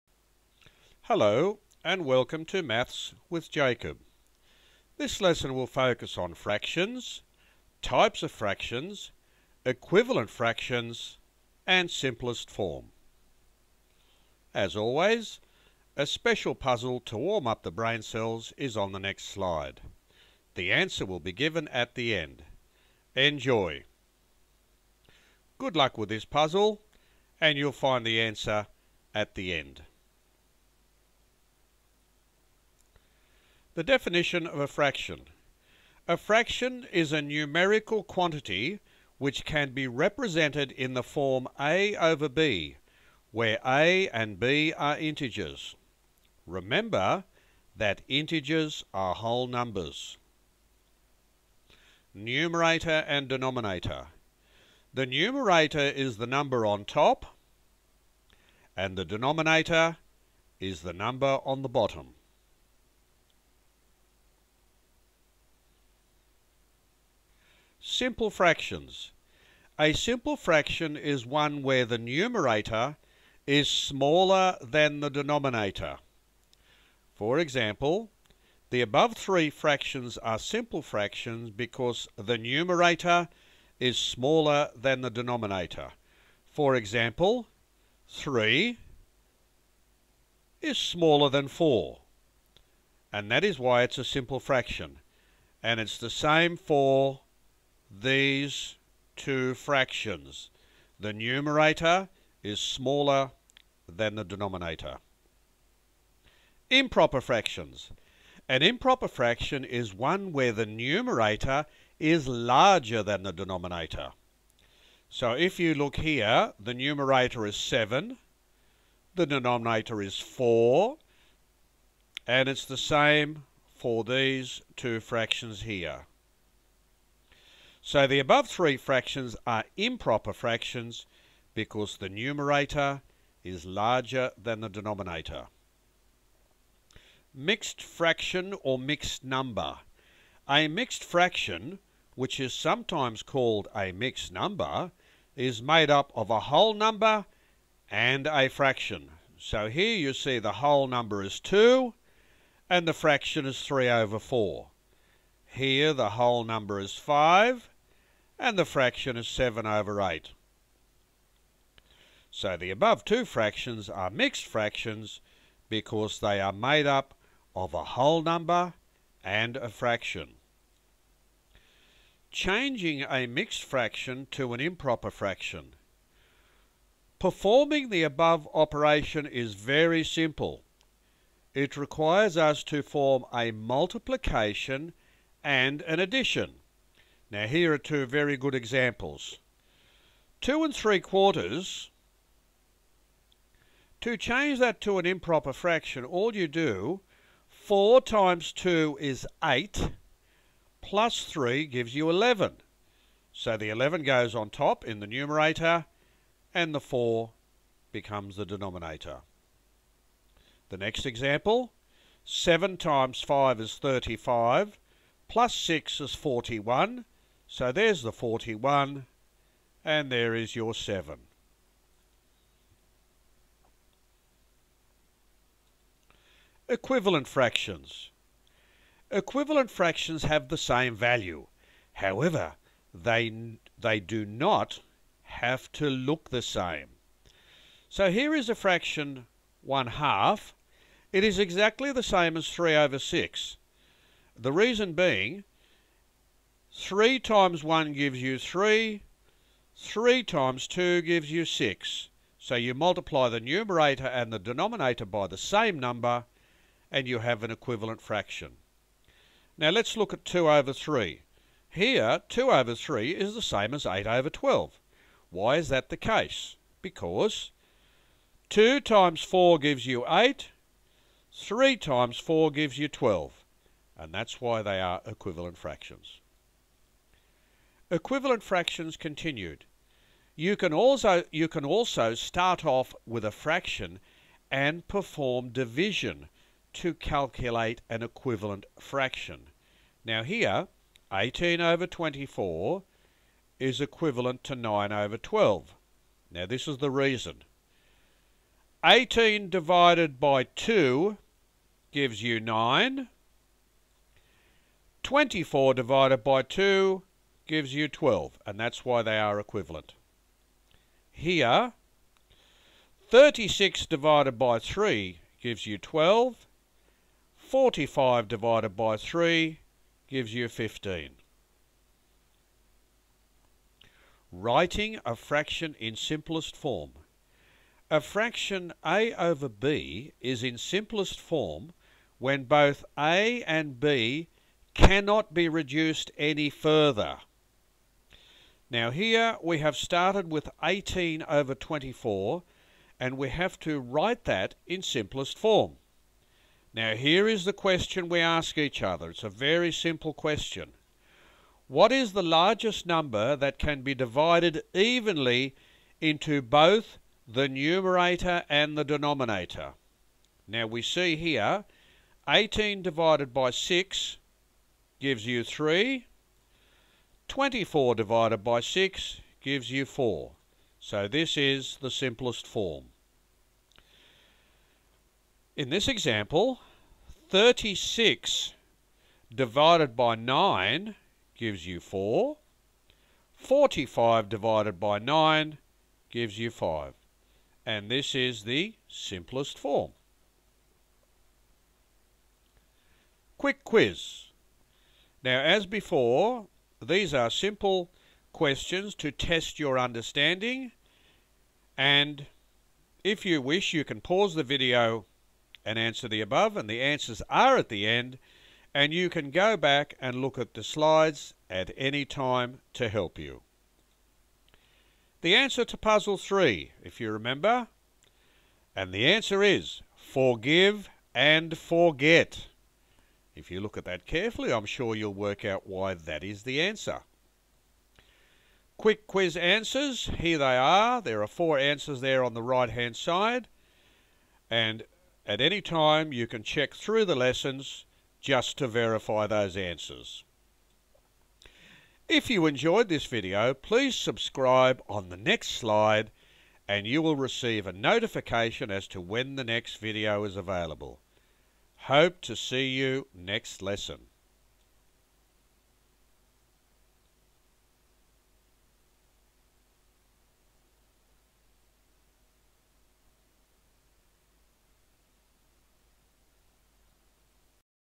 Fractions Equivalent Fractions Simplest Form
This video will look at different types of fractions, equivalent fractions and simplest form. The definition of a fraction will be given. The numerator and the denominator will be highlighted. The concept of a simple fraction will be explained. Improper fractions will also be featured. The concept of a mixed fraction or a mixed number will  be featured. We will also explore how to change a mixed fraction to an improper fraction. We will explain how to find equivalent fractions, as well as how to write a fraction in simplest form.